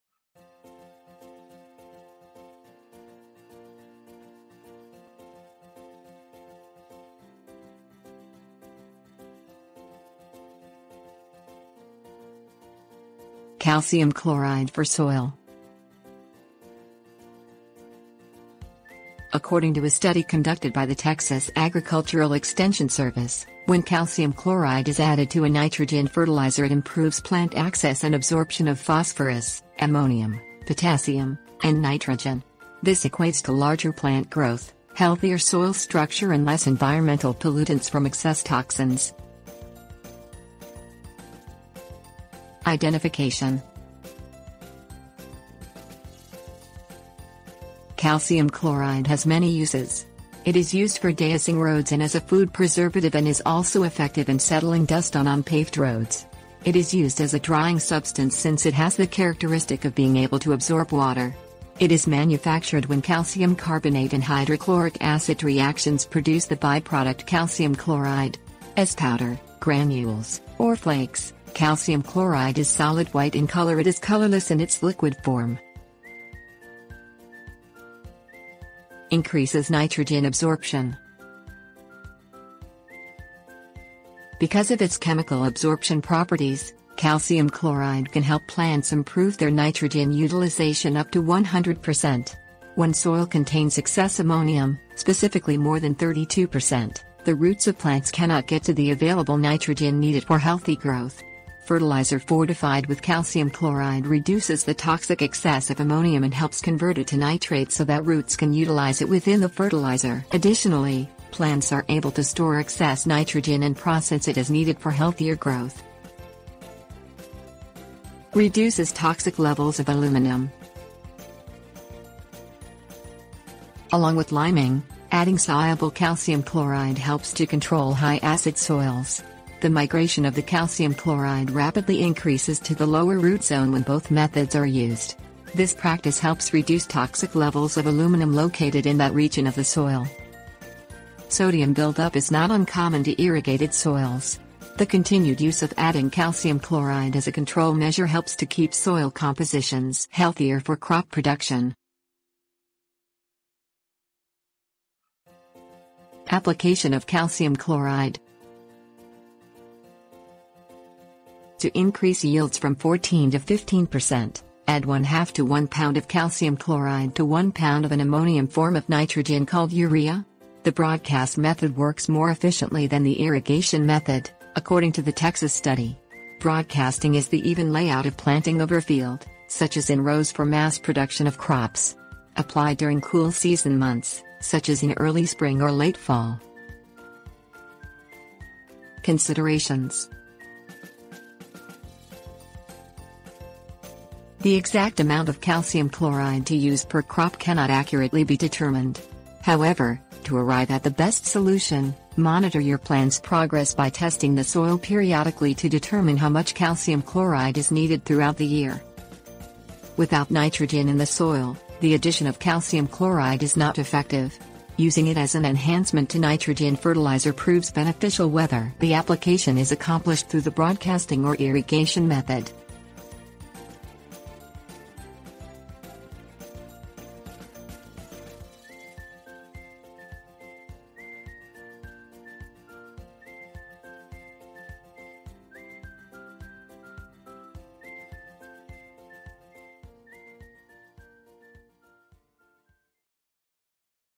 Calcium Chloride for Soil
Calcium Chloride for Soil. According to a study conducted by the Texas Agricultural Extension Service, when calcium chloride is added to a nitrogen fertilizer it improves plant access and absorption of phosphorus, ammonium, potassium, and nitrogen. This equates to larger plant growth, healthier soil structure and less environmental pollutants from...

Table of contents Calcium Chloride for Soil
Identification 00:41
Increases Nitrogen Absorption 01:17
Reduces Toxic Levels of Aluminum 01:57
Application of Calcium Chloride 02:35
Considerations 03:08